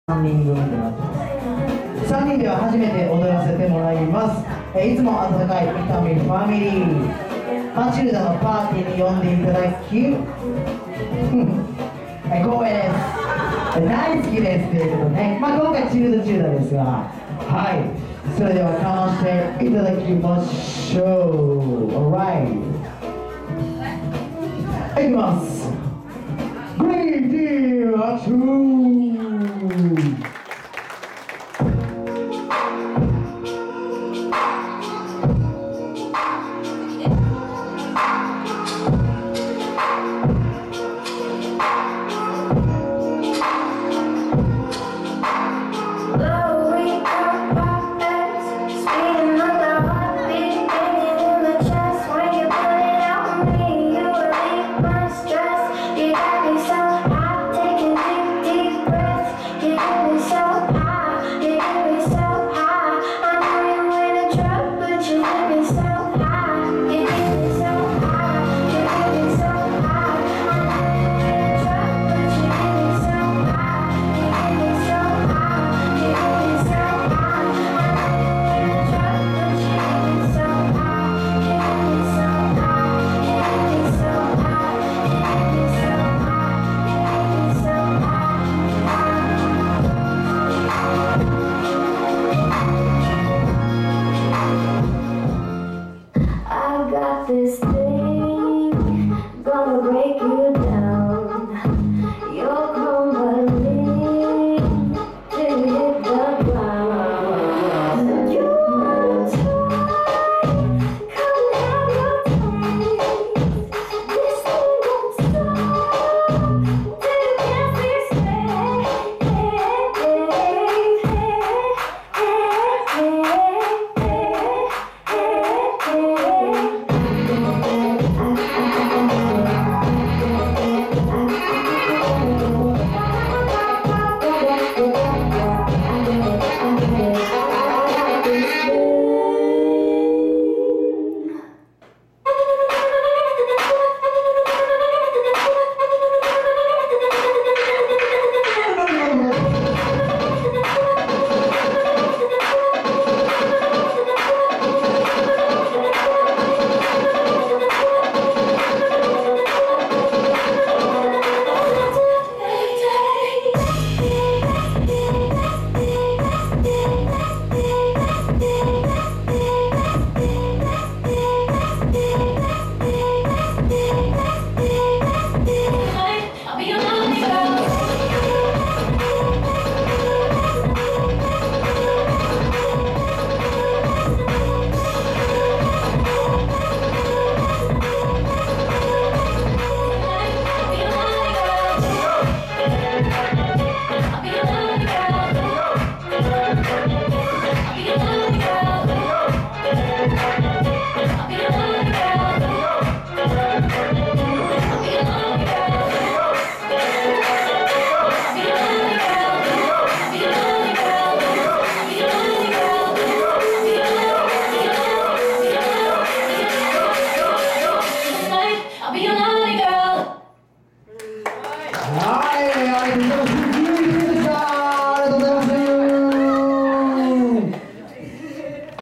Greedy《ゲストショー》~伊丹ダンスイベント/チルドチルダ vol.11 itami hall B1F 2015.06.28(sun)
伊丹のストリートダンスNO.1イベント！
アットホームな空間でダンス初心者の子達でも参加しやすい環境となっています。伊丹を­­ダンスで盛り上げようをスローガンに開催しています！
参加ご希望の方は、下記のメールアドレスまでご連絡お待ちしております。
次回の開催は9月！！！！

【タイトル】childchilda/チルドチルダvol.11 / X`M / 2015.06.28(sun)
【撮影】cyclework
【ダンサー】Greedy《ゲストショー》